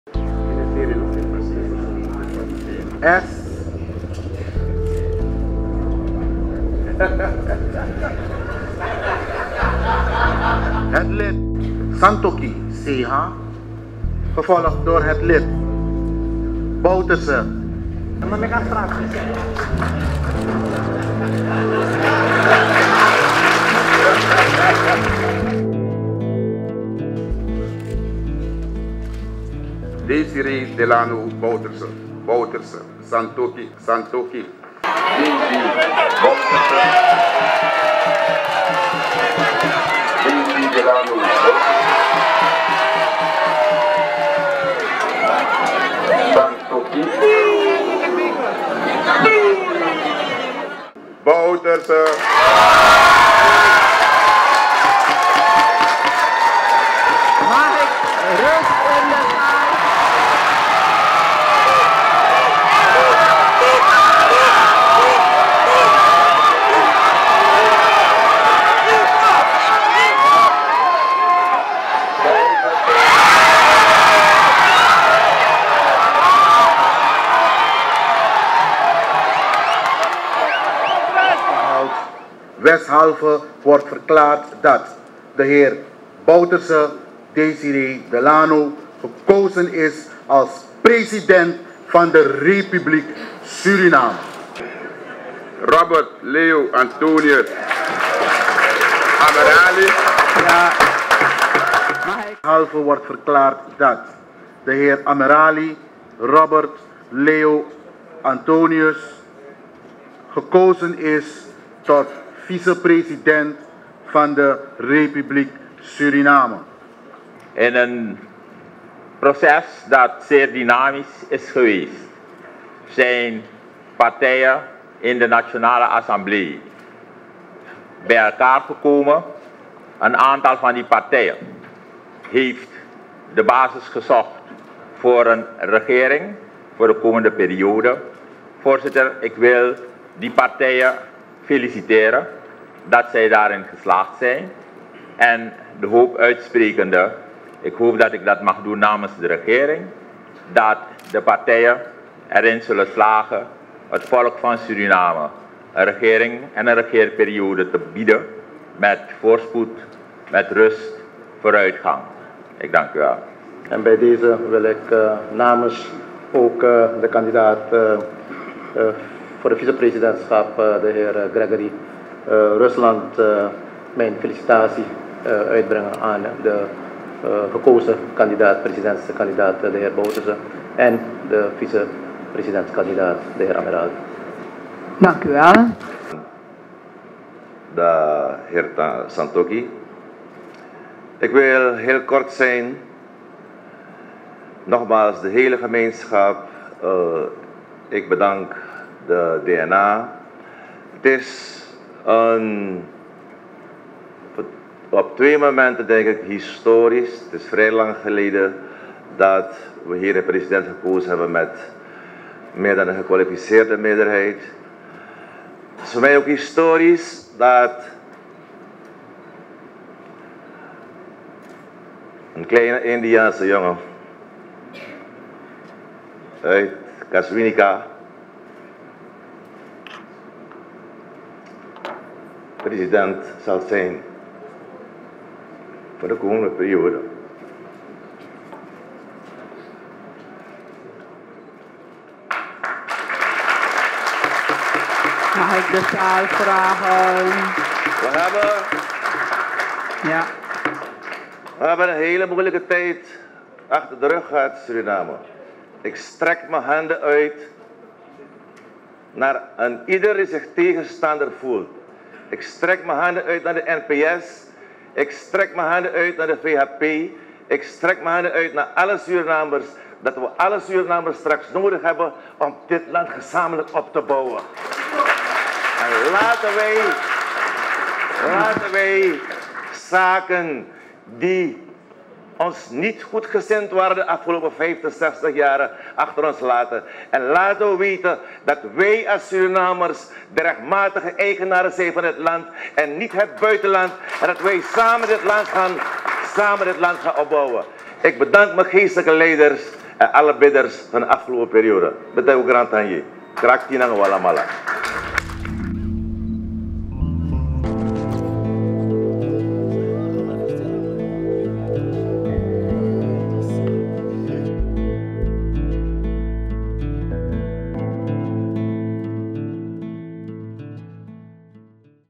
Terugblik: Bouterse gekozen tot staatshoofd
19 juli 2010
Desi Bouterse, voorzitter van de Mega Combinatie, is met 36 stemmen gekozen tot staatshoofd van de republiek Suriname. Bouterse pleegde op 25 februari 1980 een staatsgreep en was zeven jaar aan de macht. In 1987 werd hij 'weggestemd'. Dertig jaar later wist hij 23 zetels in de wacht te slepen. Een samenwerking met A-Combinatie en Volks Alliantie, zorgde ervoor dat hij tweederde meerderheid haalde in het parlement. Bouterse noemde zijn verkiezing een 'historisch moment': "Een kleine indiaanse jongen van Kasiwinika is gekozen tot president van de Republiek Suriname".